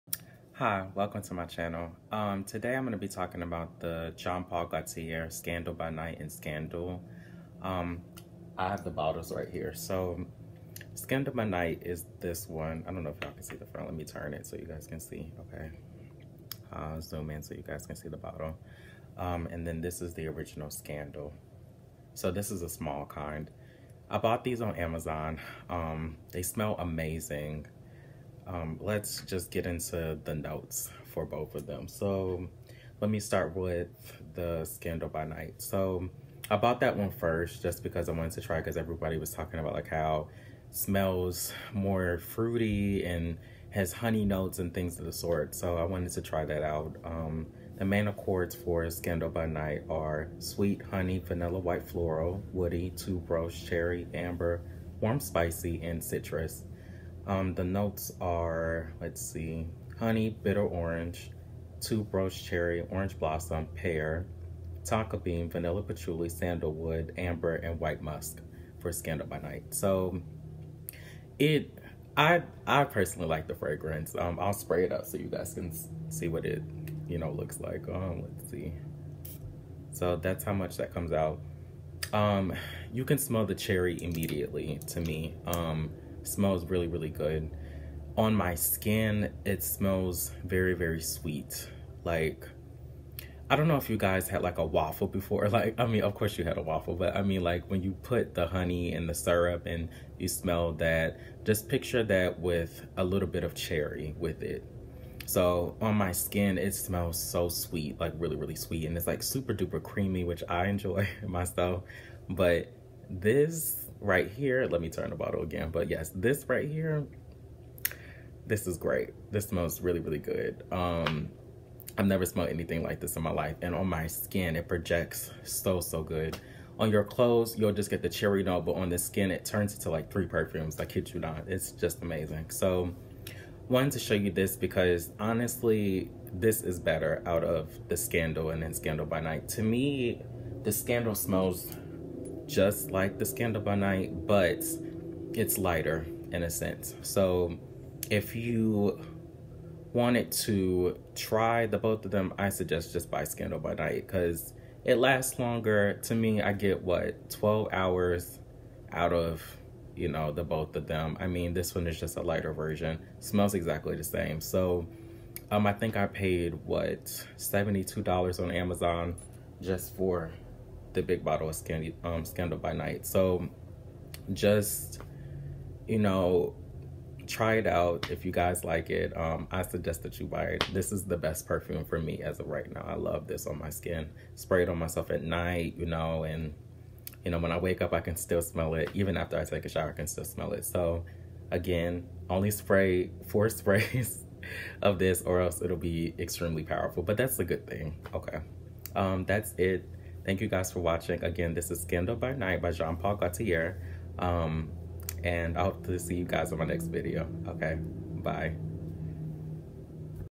Jean Paul Gaultier Scandal and Scandal By Night
* I’ll be talking About these Fragrances in the video Scandal and Scandal by night by Jean Paul Gaultier.

If you want more information on the fragrances I talked about, here’s a site describing the notes

A list of sites to purchase fragrances from